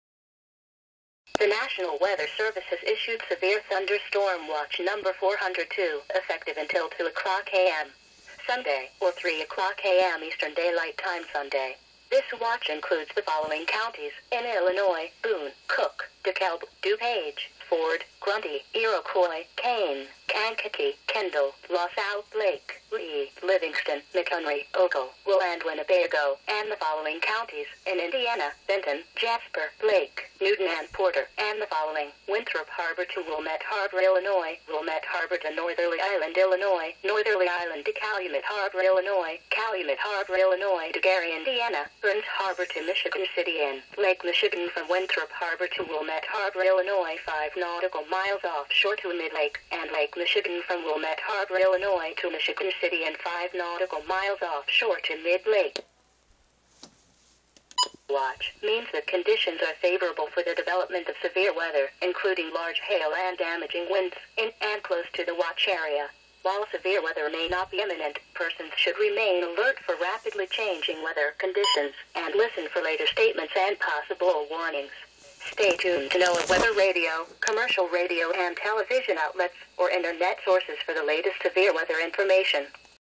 EAS Alert #138- Severe Thunderstorm Watch 6/16/12
I received 10 alerts on this day. This is SVA #2. I did miss the inital coded message (thats why the WR-100 times out).